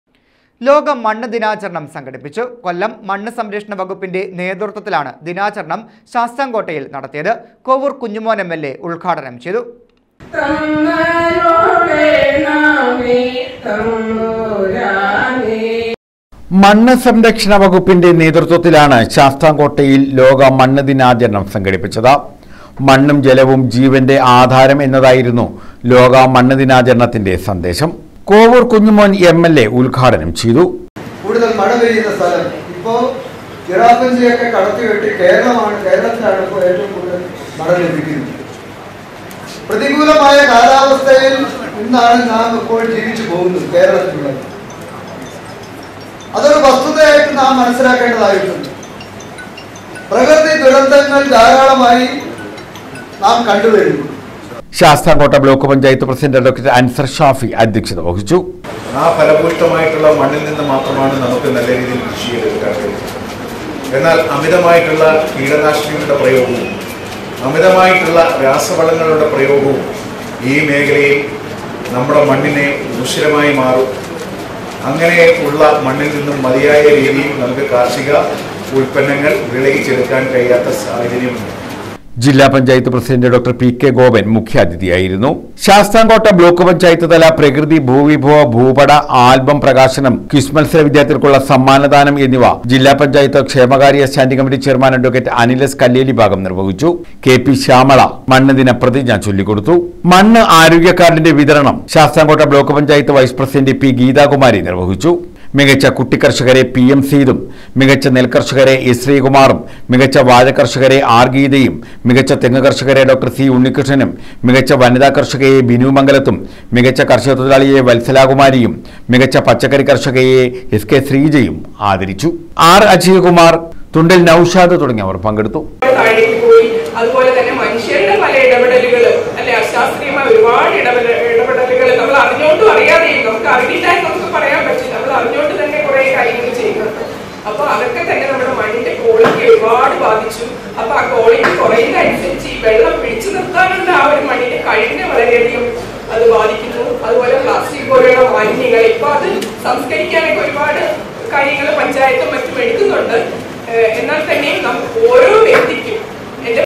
ലോക മണ്ണ് ദിനാചരണം
ലോക മണ്ണ് ദിനാചരണം സംഘടിപ്പിച്ചു. കൊല്ലം മണ്ണ് സംരക്ഷണ വകുപ്പിന്റെ നേതൃത്വത്തിലാണ് ദിനാചരണം ശാസ്താംകോട്ടയില്‍ നടത്തിയത്. കോവൂര്‍ കുഞ്ഞുമോന്‍ എം.എല്‍.എ ഉദ്ഘാടനം ചെയ്തു.